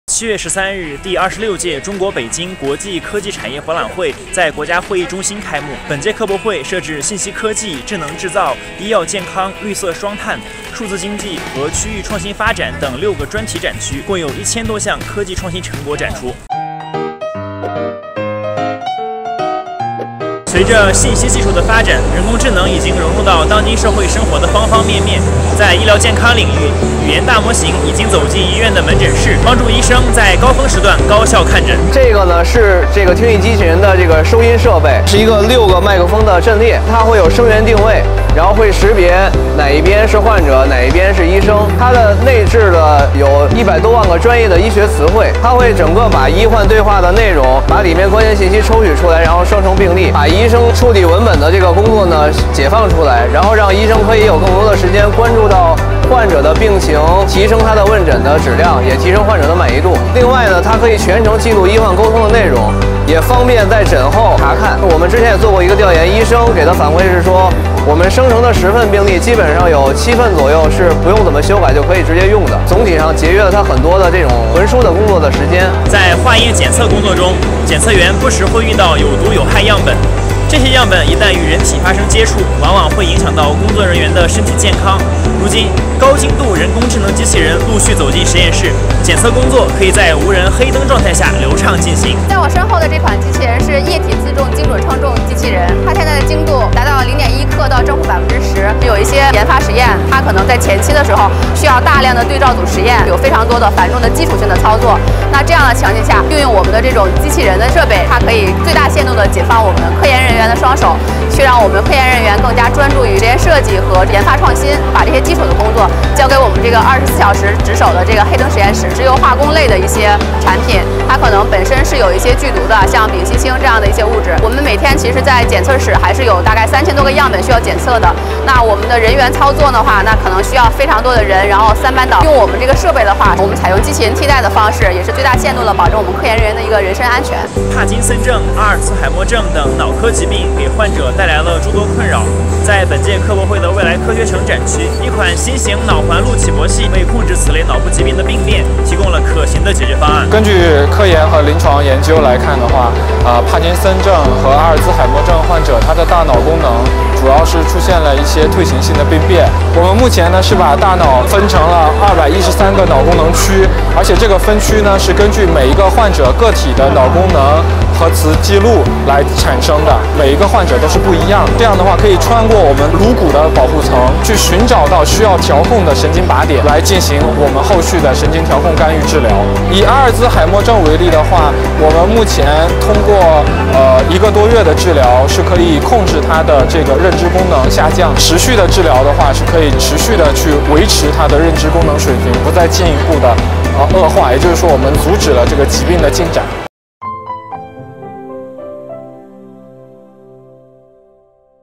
医生专心问诊AI自动录病历 多项前沿科技亮相北京科博会
7月13日，第26届中国北京国际科技产业博览会在国家会议中心开幕。本届科博会设置信息科技、智能制造、医药健康、绿色双碳、数字经济和区域创新发展等6个专题展区，共有1000多项科技创新成果展出。随着信息技术的发展，人工智能已经融入到当今社会生活的方方面面。在医疗健康领域，语言大模型已经走进医院的门诊室，帮助医生在高峰时高效看诊，据展会上北京一家科技公司工作人员介绍，该公司开发的听译机器人内置有大量医学词汇的基础语料，可以识别医患对话的内容并提取关键信息自动生成病历，把医生从处理文本的工作中解放出来，更加专注于问诊工作。（谢龙飞）